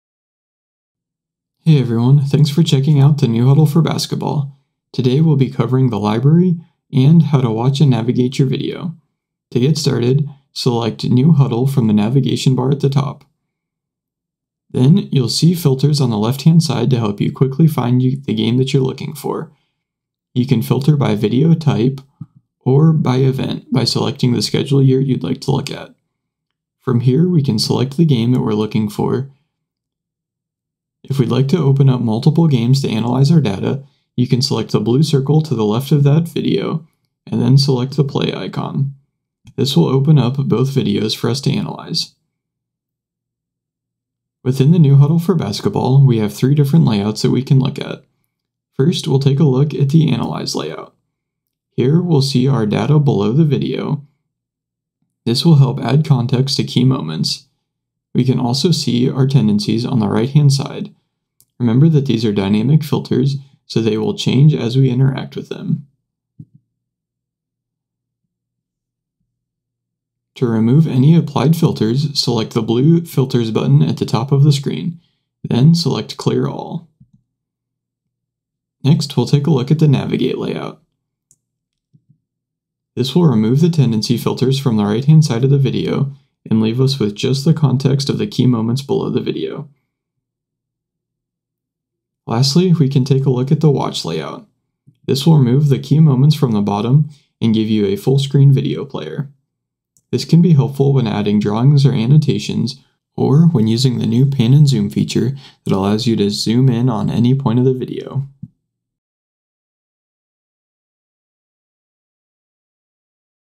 Navigate Your Film | Basketball
Check out the new Hudl library. We will also cover how to watch and manage your film.